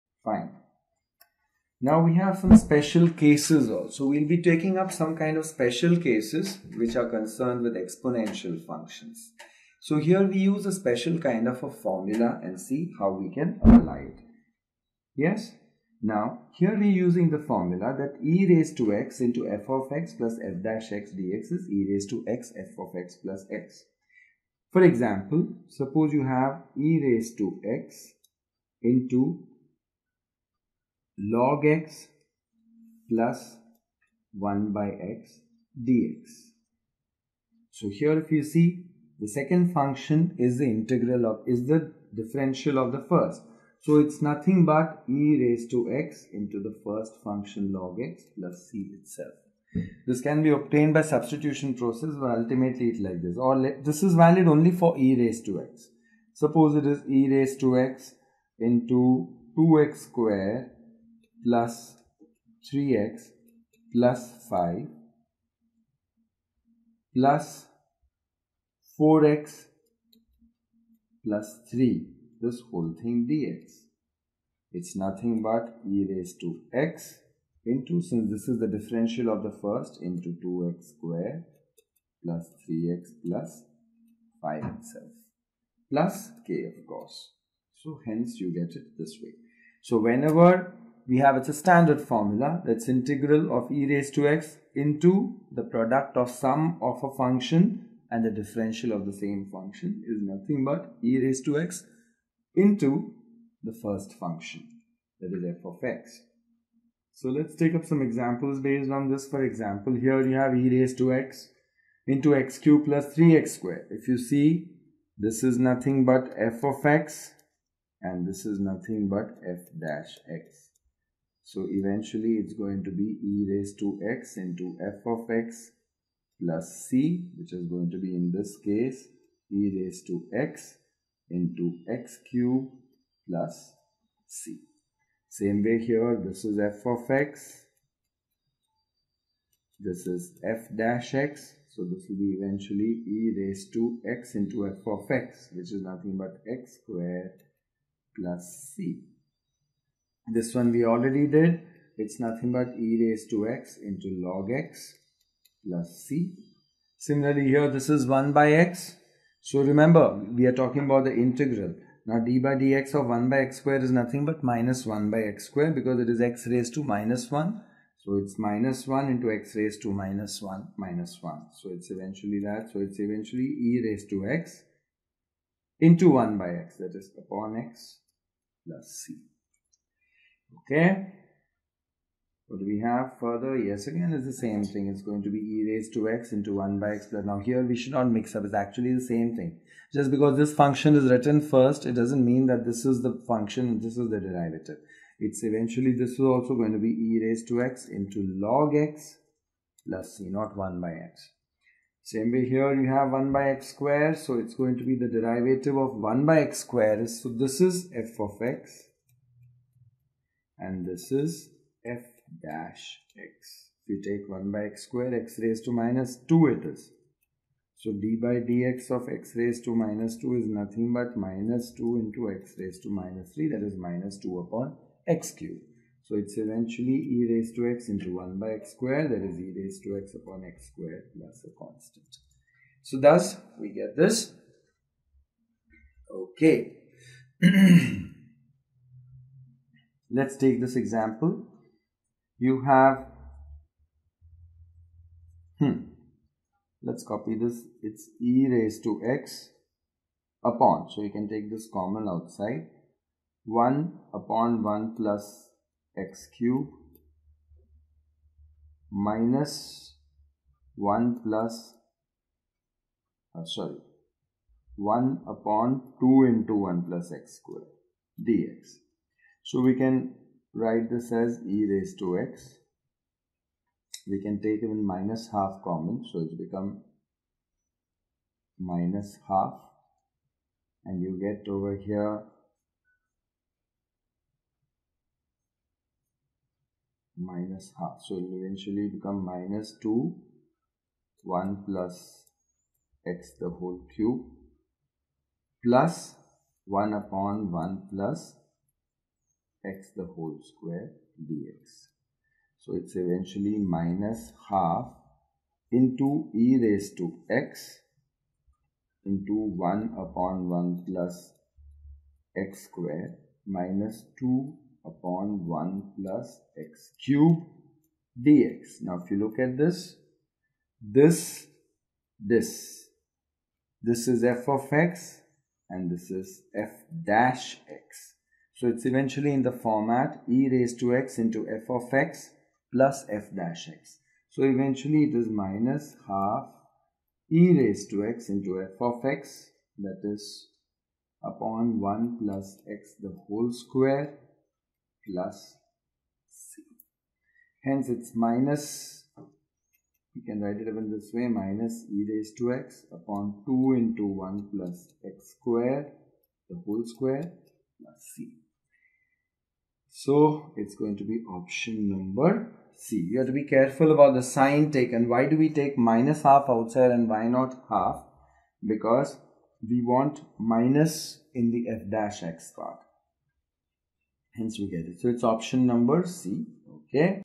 Special cases- Integration by Parts | CA CPT | CS & CMA Foundation | Class 11
Special cases- Integration by Parts, Learn Integration, What is Integration? Basic Formulas for Integration, Handling Special variable in Integration, What is Partial Fraction? What is Definite Integration.